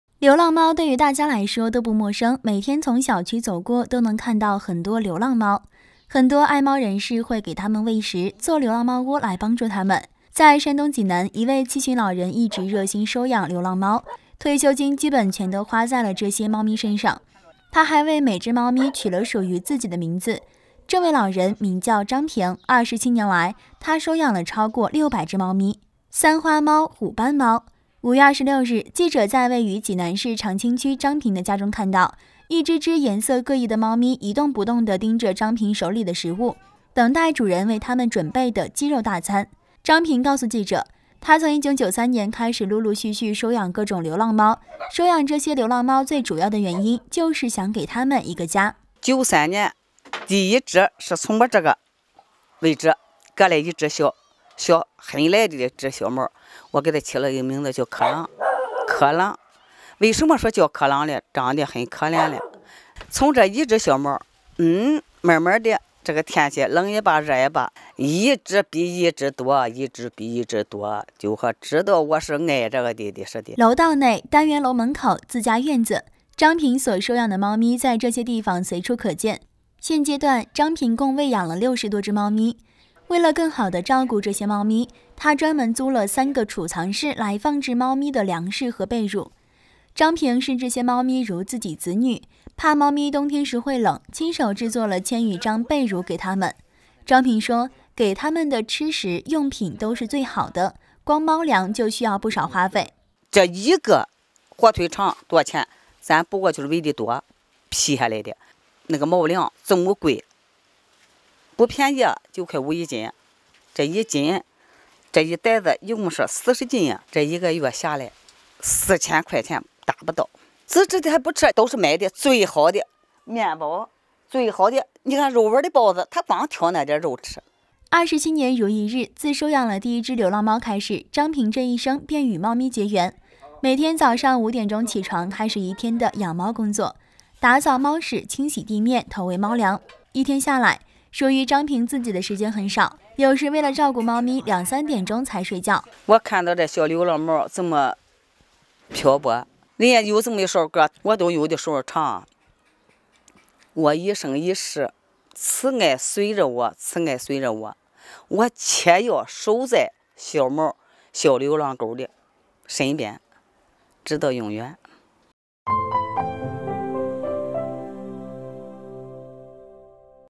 七旬老人27年养600余只流浪猫：想给它们一个家
流浪猫对于大家来说都不陌生，每天从小区走过都能看到很多流浪猫，很多爱猫人士，会给它们喂食，做流浪猫窝来帮助它们。在山东济南，一位七旬老人一直热心收养流浪猫，退休金基本全都花在了这些猫咪身上，她还为每只猫咪取了属于自己的名字。这位老人名叫张平，27年来，她收养了超过600只猫咪。